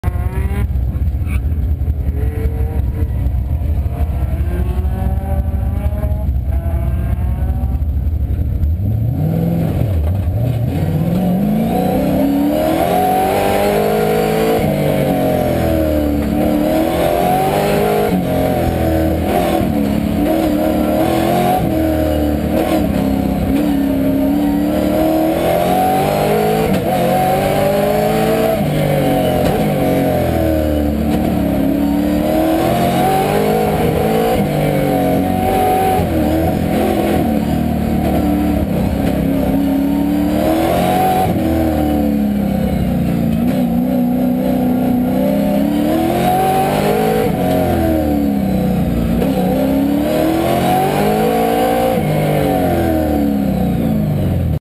Brian Hobaugh AAS Marina July 2012
American Autocross Series, Brian Hobaugh 1973 Camaro